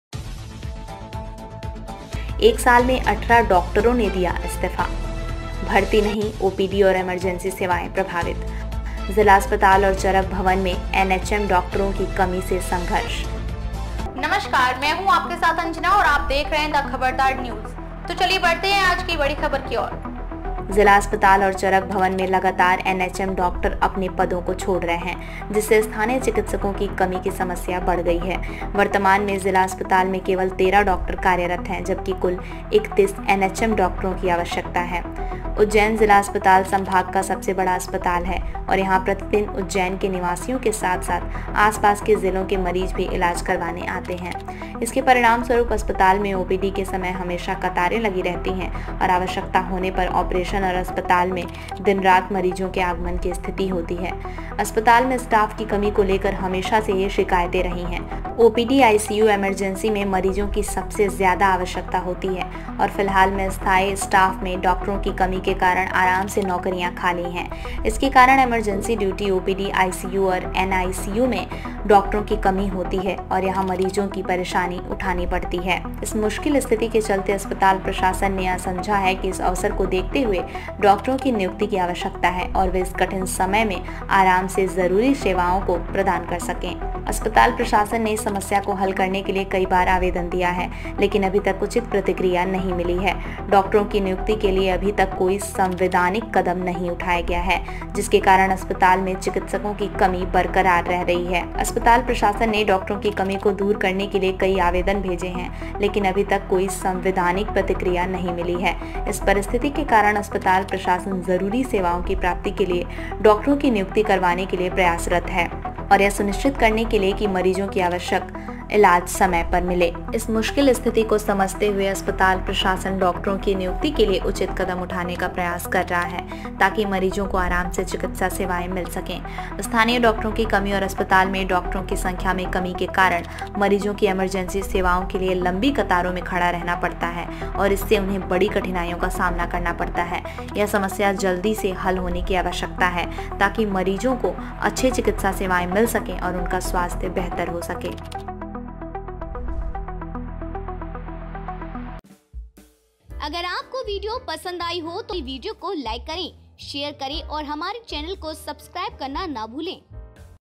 Ujjain में NHM डॉक्टरों के इस्तीफे से स्वास्थ्य संकट गहराया। Ujjain News। MP NEWS
Ujjain News- उज्जैन, मध्यप्रदेश: जिला अस्पताल और चरक भवन में लगातार एनएचएम डॉक्टर अपने पदों को छोड़ रहे हैं, जिससे स्थानीय चिकित्सकों की कमी की समस्या बढ़ गई है। वर्तमान में जिला अस्पताल में केवल 13 डॉक्टर कार्यरत हैं, जबकि कुल 31 एनएचएम डॉक्टरों की आवश्यकता है। उज्जैन जिला अस्पताल संभाग का सबसे बड़ा अस्पताल है और यहां प्रतिदिन उज्जैन के निवासियों के साथ-साथ आसपास के जिलों के मरीज भी इलाज करवाने आते हैं। इसके परिणामस्वरूप अस्पताल में ओपीडी के समय हमेशा कतारें लगी रहती हैं और आवश्यकता होने पर ऑपरेशन और अस्पताल में दिन-रात मरीजों के आगमन की स्थिति होती है। अस्पताल में स्टाफ की कमी को लेकर हमेशा से ही शिकायतें रही हैं। ओपीडी, आईसीयू, इमरजेंसी में मरीजों की सबसे ज्यादा आवश्यकता होती है, और फिलहाल में स्थायी स्टाफ में डॉक्टरों की कमी के कारण आराम से नौकरियाँ खाली हैं। इसके कारण इमरजेंसी ड्यूटी, ओपीडी, आईसीयू, और एनआईसीयू में डॉक्टरों की कमी होती है और यह मरीजों को परेशानी उठानी पड़ती है। इस मुश्किल स्थिति के चलते, अस्पताल प्रशासन ने यह समझा है कि इस अवसर को देखते हुए डॉक्टरों की नियुक्ति की आवश्यकता है और वे इस कठिन समय में आराम से जरूरी सेवाओं को प्रदान कर सकें।